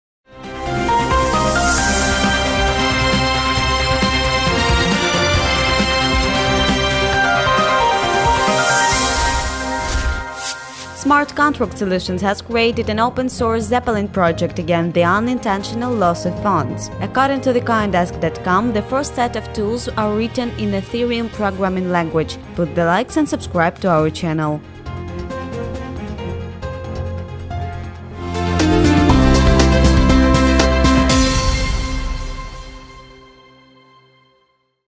KCN News: Zeppelin project by Smart Contract Solutions on Ethereum language
Smart Contract Solutions has created an open-source Zeppelin project against the unintentional loss of funds. According to the Coindesk.com, the first set of tools are written in Ethereum programming language.
BTC: 1JdRXQuqicwSyn61cd9jyXzAk2QUfu4KEW
EDR: koles-tv